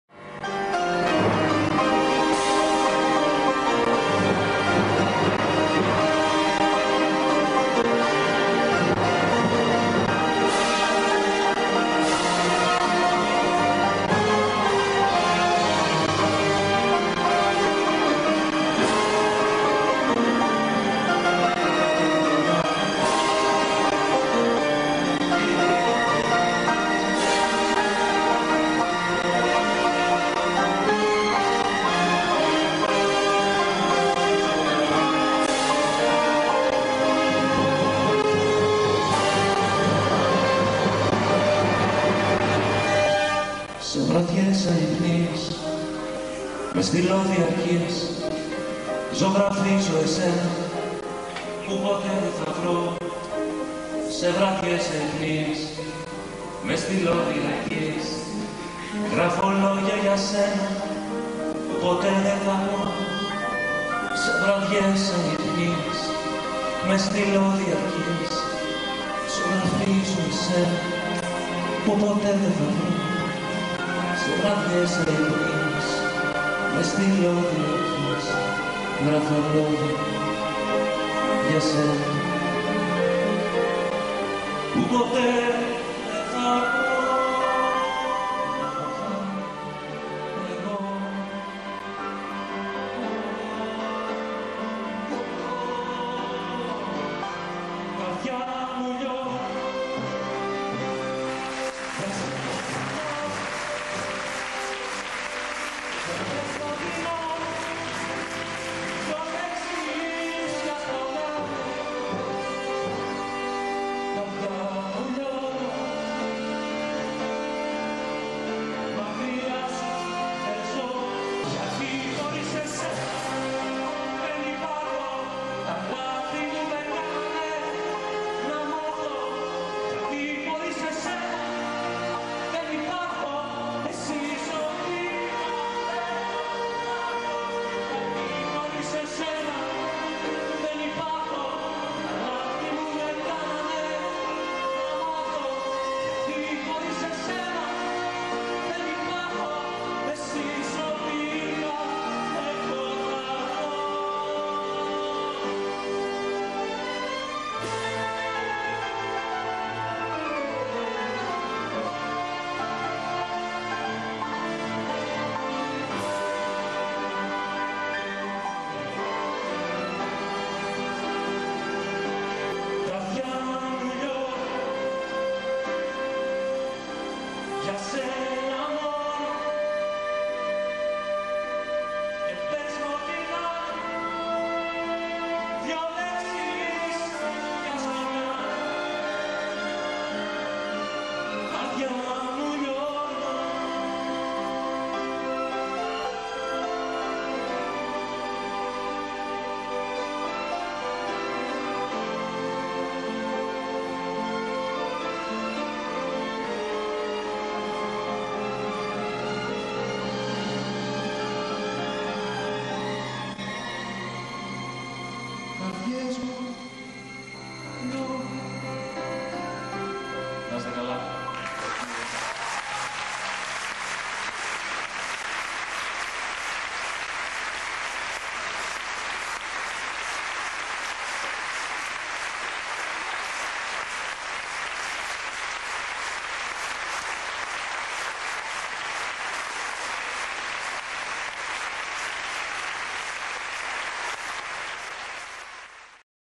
Στεφανος Κορκολης -- Συμφωνικη Ορχηστρα Βολου
ο Στέφανος Κορκολής με τη Συμφωνική Ορχήστρα Βόλου το 1997,σ΄ένα Medley με αγαπημένα τραγούδια...